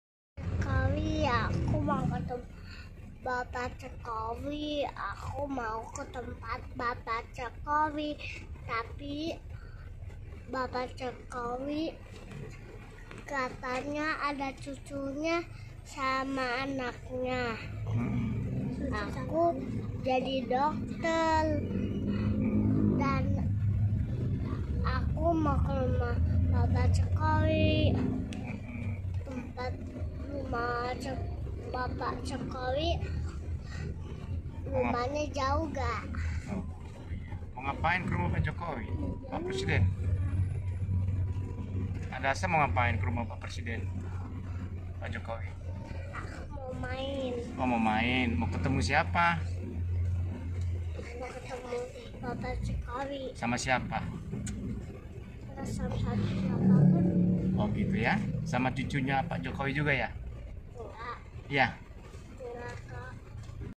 Hadasa umur 3 Tahun Rindu Bertemu Presiden Bapak Jokowidodo
Kerinduan Hadasa usia 3 Tahun bertemu Presiden bapak Jokowidodo begitu luar biasa, disampaikan saat melakukan perjalanan pulang. Hadasa mengungkapkan kerinduannya adalah murni tanpa ada setingan, diatur & diajari.